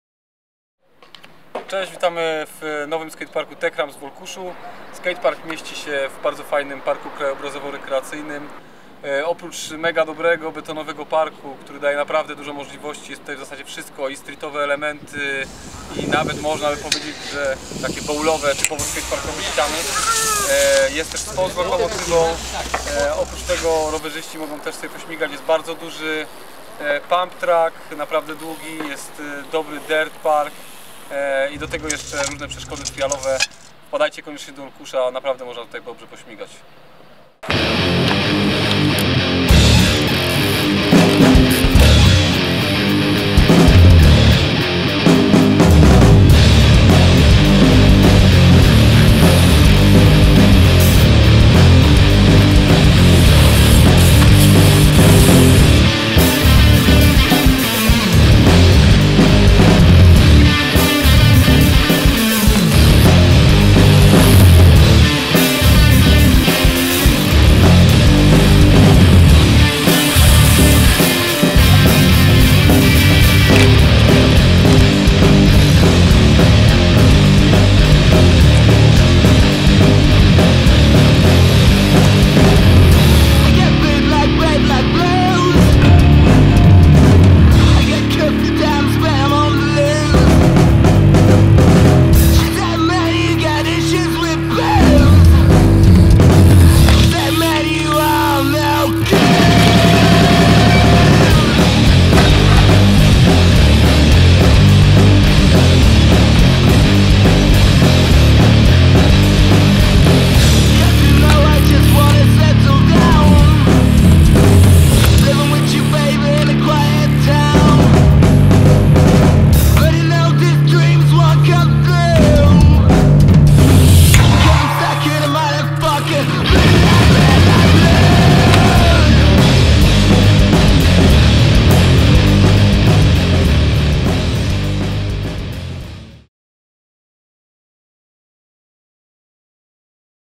Skatepark Review - Olkusz/BMX
Sprawdźcie nowy skatepark Techramps w Olkuszu, będący częścią parku krajobrazowo - rekreacyjnego.

Jeżdżą: Piotr "Leszczu" Leszczyński i Michał Jaworski.

Check out the new skatepark Techramps in Olkusz, part of the  landscape - recreational park.

Riders:
Piotr "Leszczu" Leszczyński i Michał Jaworski.